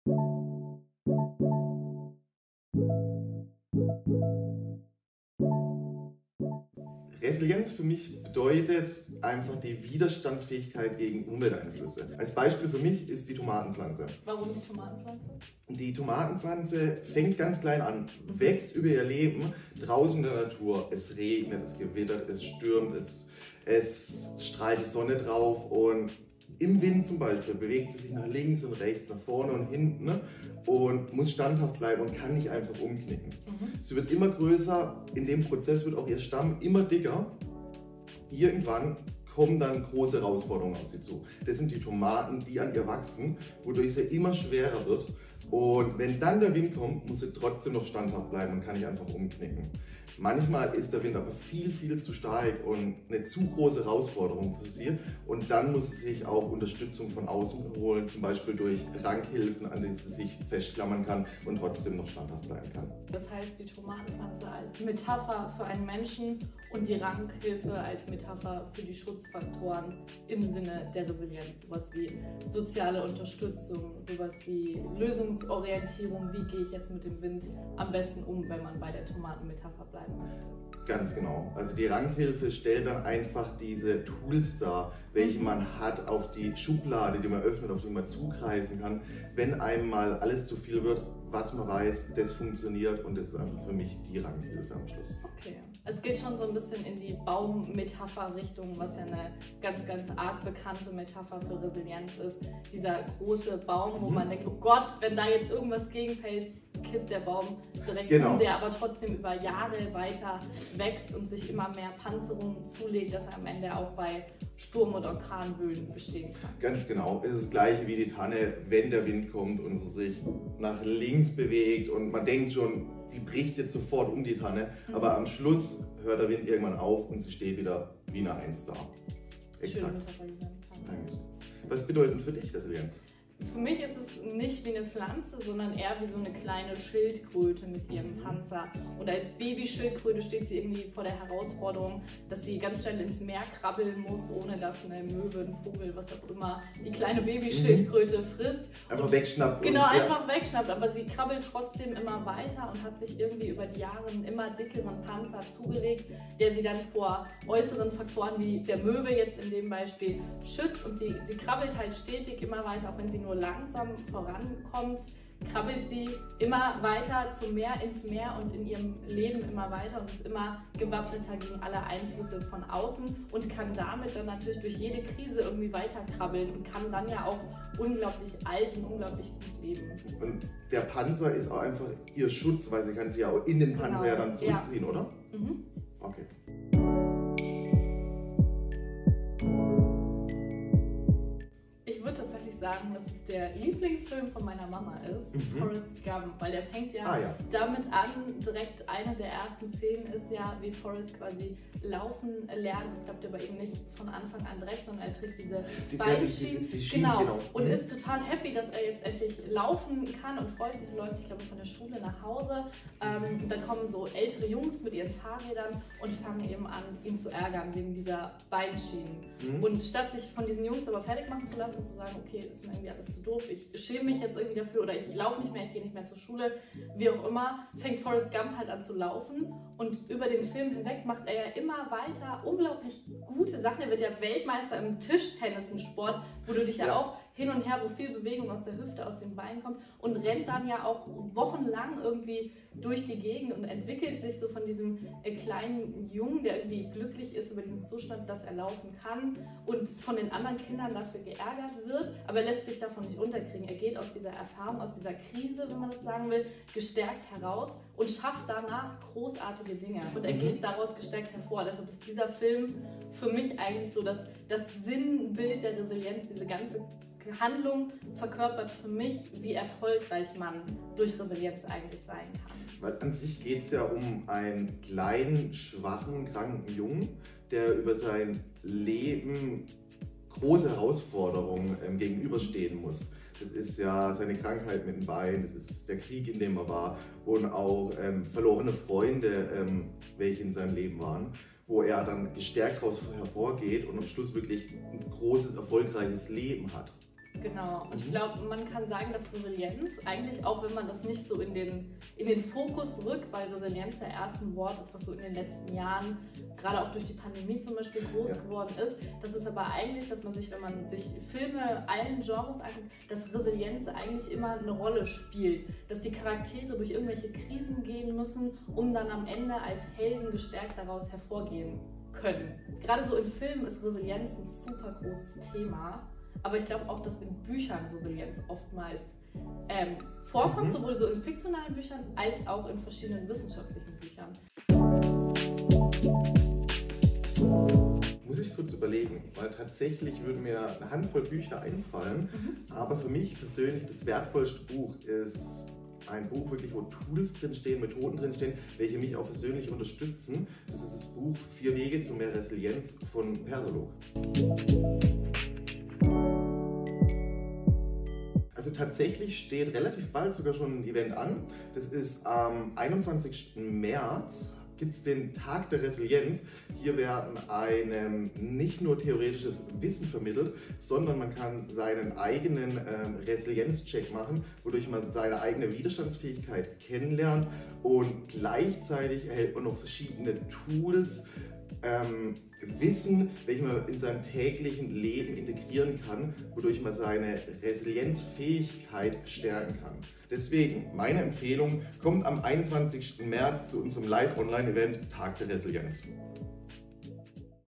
Der persolog® Resilienz-Mini-Podcast | Mit Jana & Julian
Was Resilienz mit einer Tomaten-Pflanze und einer Schildkröte verbindet, das erfährst du in der heutigen Mini-Podcast-Folge mit Jana und Julian!

Wenn du dich mehr mit der Stärkung deiner eigenen Resilienz beschäftigen willst, dann ist unsere kostenfreie Veranstaltung "Tag der Resilienz" genau das richtige für dich.